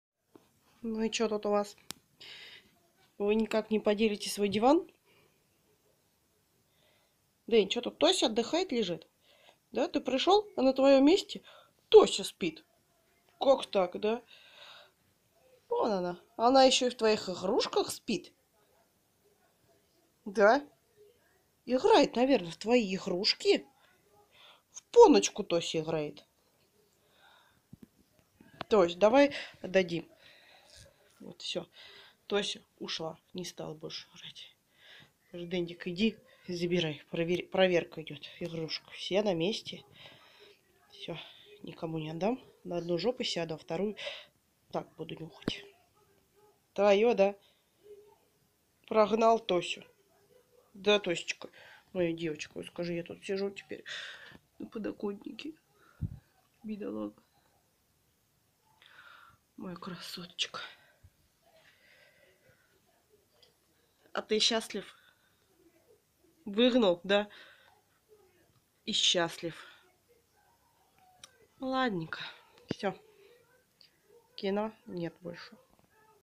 Пекинес Дендик и кошка Тося
Жадина пекинес всегда ревнует когда кошка находится рядом с его игрушками